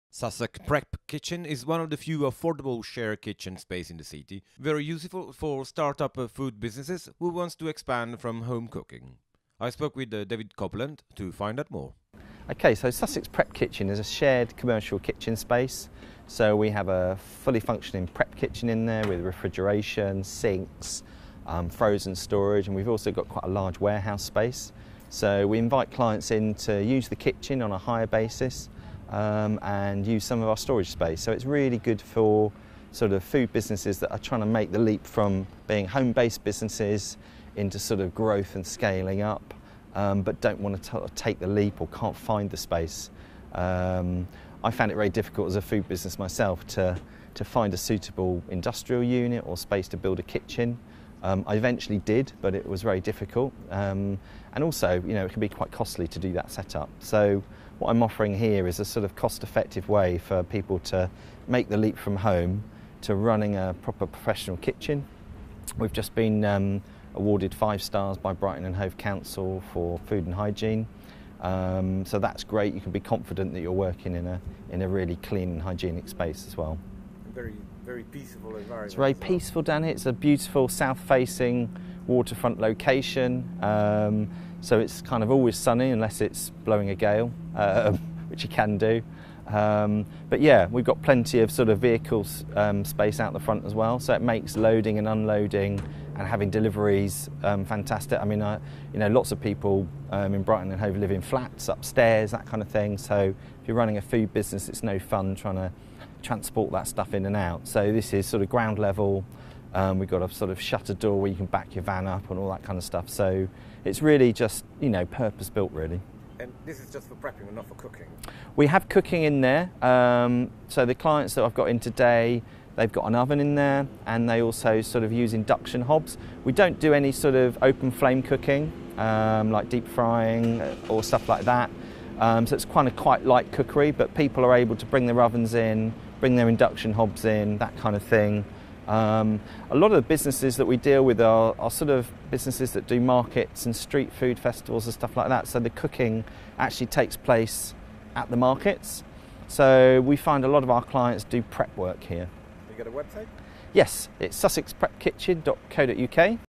Sussex Prep Kitchen
Sussex Prep Kitchen is one of the few affordable shared kitchen space the city, very useful for start up food businesses who wants to expand from home cooking.
William Ranieri spoke with David Copeland to find out more.
William Ranieri reports